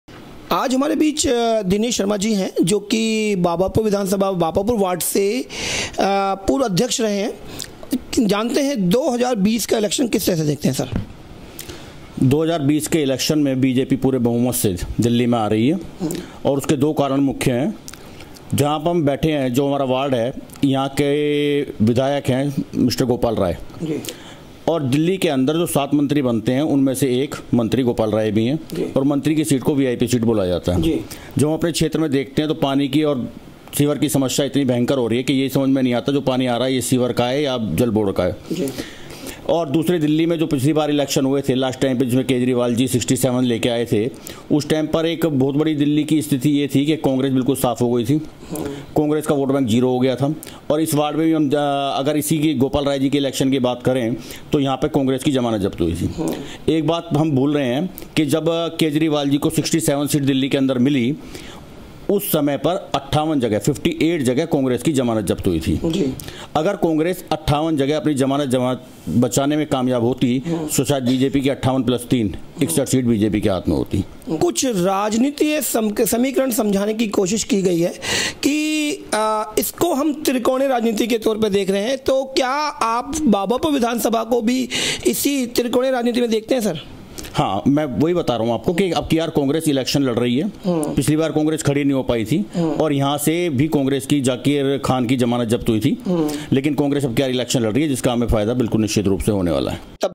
Dinesh Sharma BJP
Dinesh Sharma Ji from BJP shared his views on election 2020